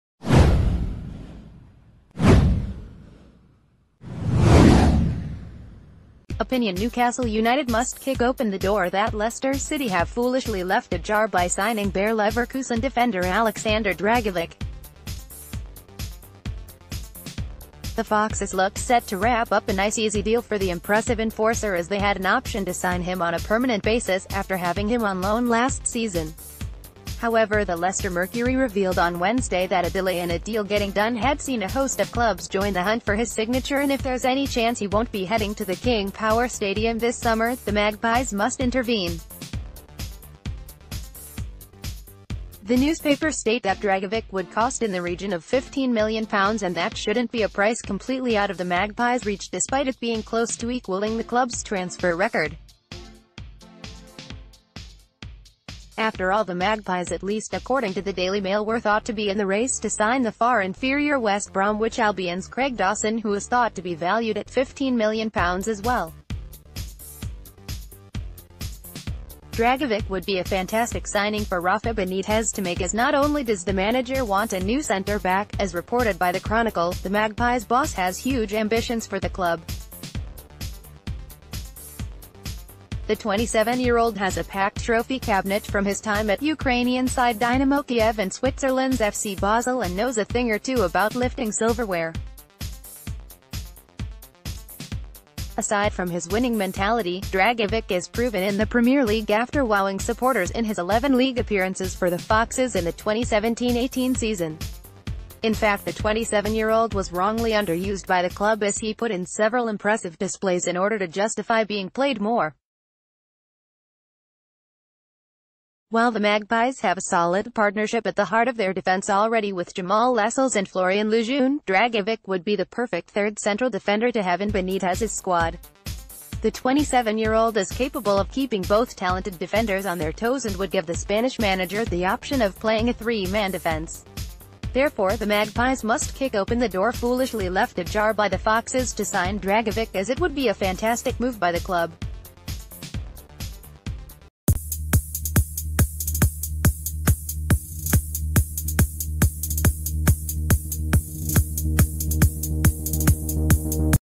Newcastle must kick open door reportedly left foolishly ajar to make fantastic enforcer signing
OPINION Newcastle United must kick open the door that Leicester City have foolishly left ajar by signing Bayer Leverkusen defender Aleksandar Dragovic. The Foxes looked set to wrap up a nice easy deal for the impressive enforcer as they had an option to sign him on a permanent basis after having him on loan last season. However the Leicester Mercury revealed on Wednesday that a delay in a deal getting done had seen a host of clubs join the hunt for his signature and if there’s any chance he won’...